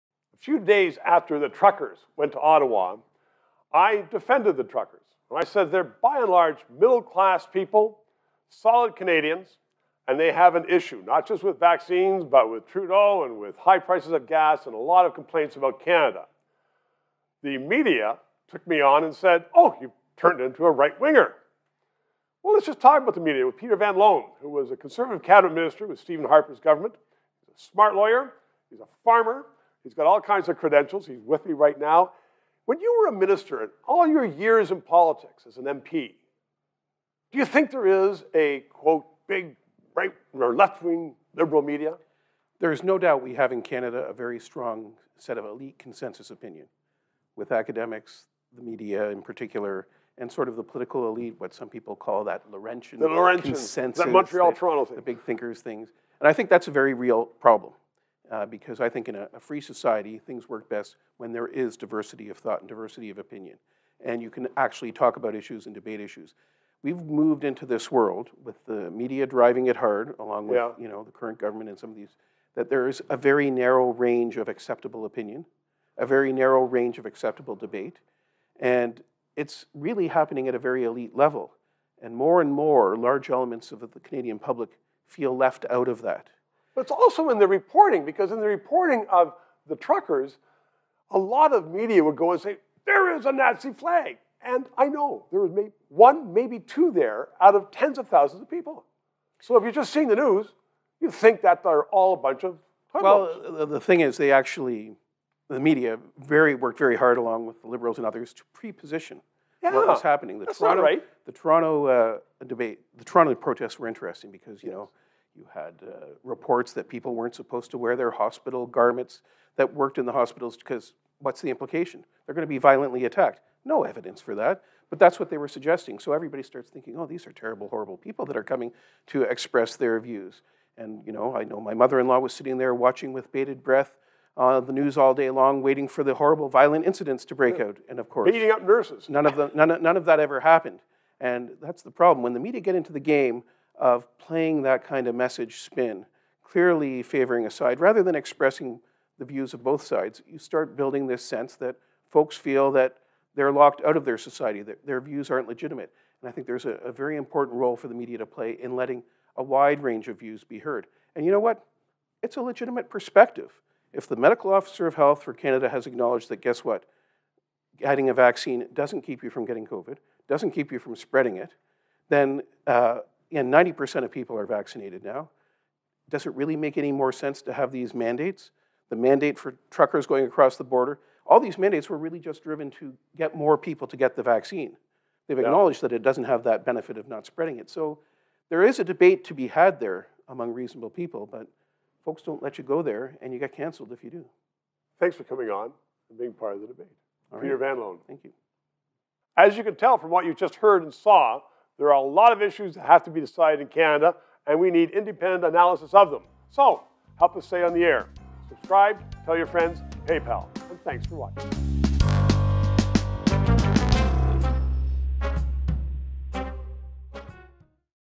Is There A Problem With Liberal Bias In The Canadian Media?
It seems that Canada is more divided than ever. Now that every major media outlet is on Justin Trudeau's payroll is there any media left to hold the government to account? By the way, this channel is not financed by the government. Peter Van Loan was a Cabinet Minister in Stephen Harper's government, he's a lawyer, and a farmer. He has some thoughts about the relationship between government and media. He joins Stephen LeDrew for Three Minutes.

Cheques accepted at:
Stephen LeDrew
303 Bay Street
Toronto, Ontario
M5H 2R1